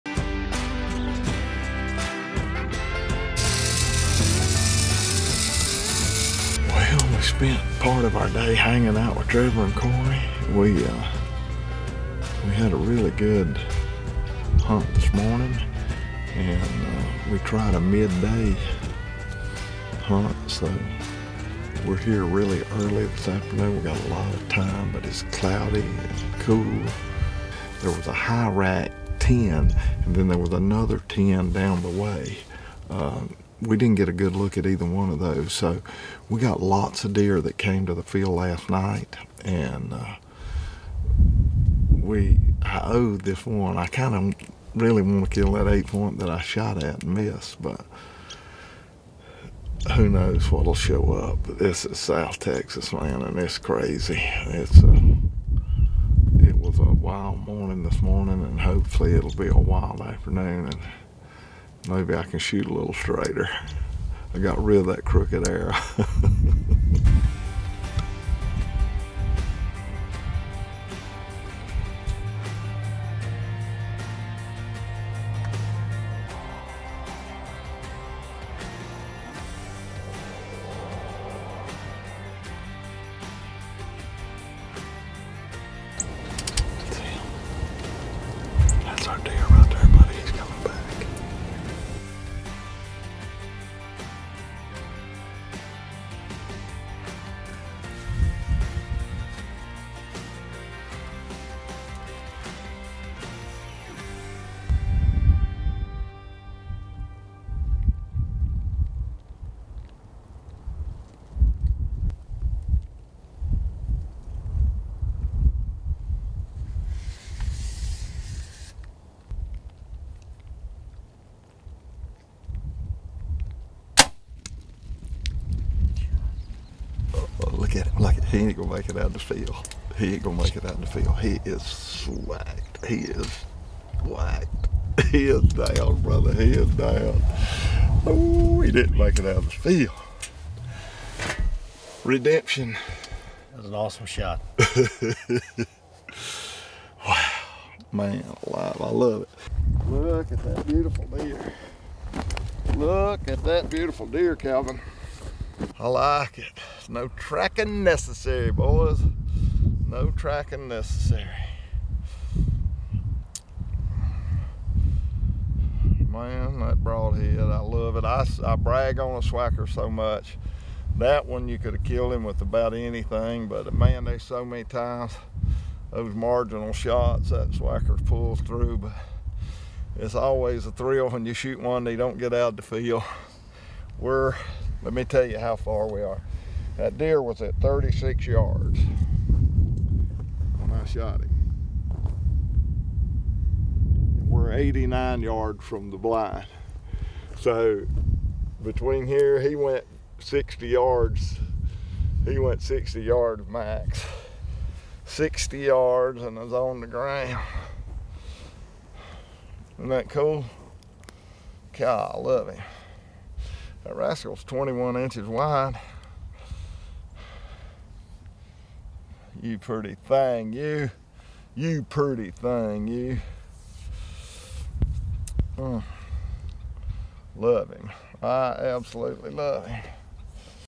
Giant 8 point
Im in Texas at the 888 Ranch and I'm like a kid in a candy store. I am fired up to be here. Hope y'all enjoy this 5 minute hunt!